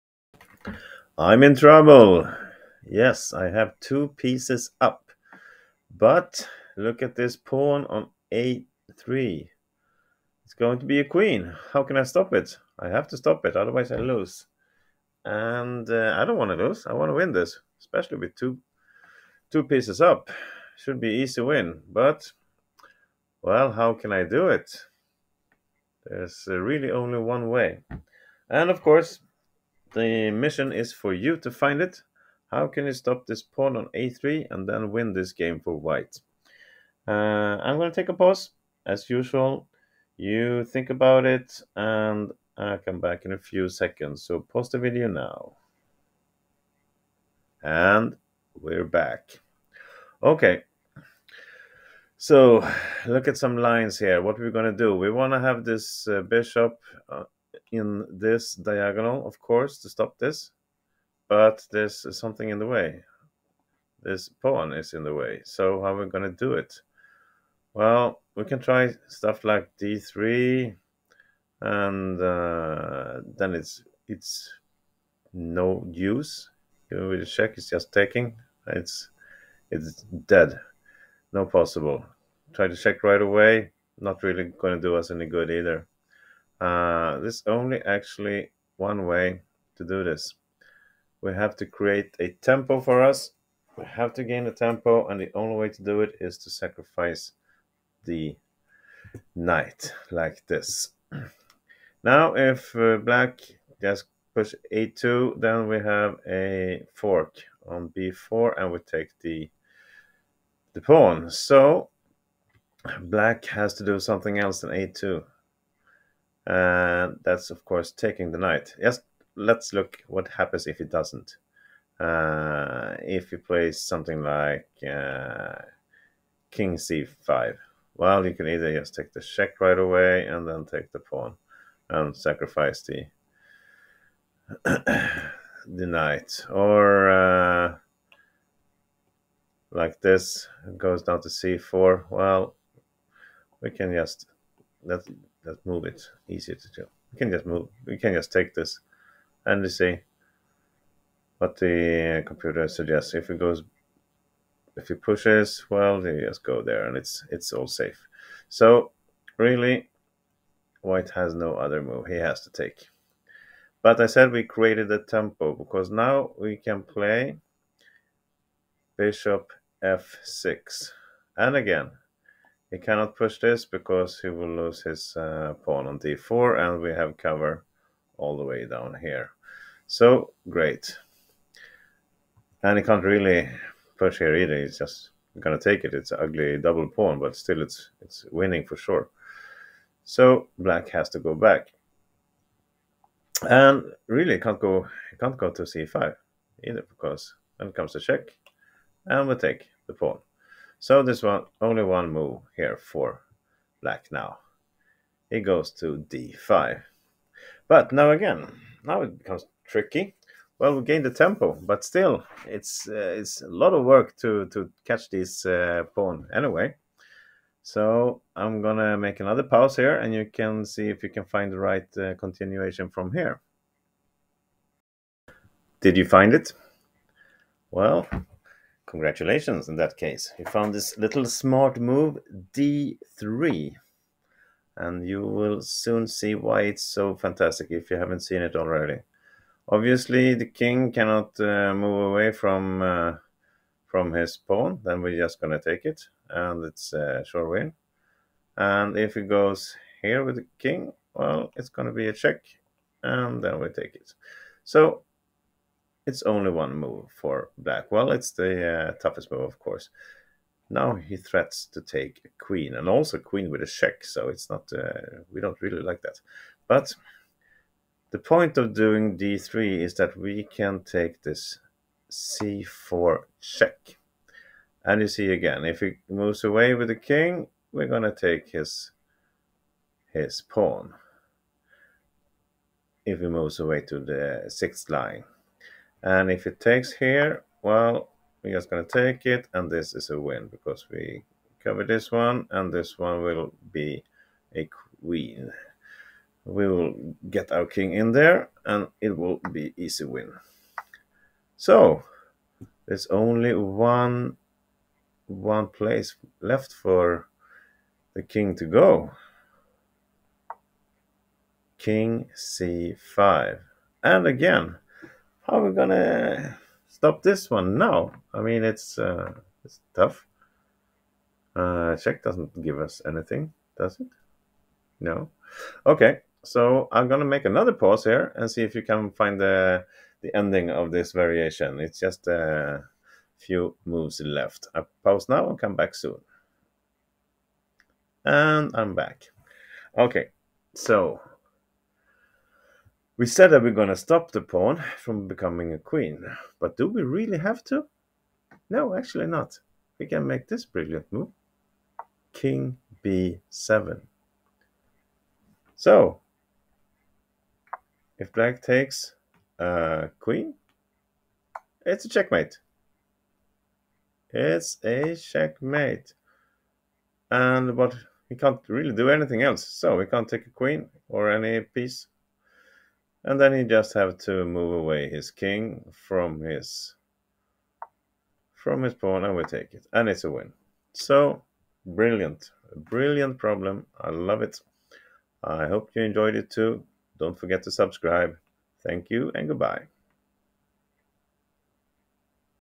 Stop the pawn!!
We are two pieces up but how can we stop blacks pawn from promote to a queen??

This chess problem is really nice and has a beautiful solution to it. Take a look!

Composed by Leonid Kubbel in 1922.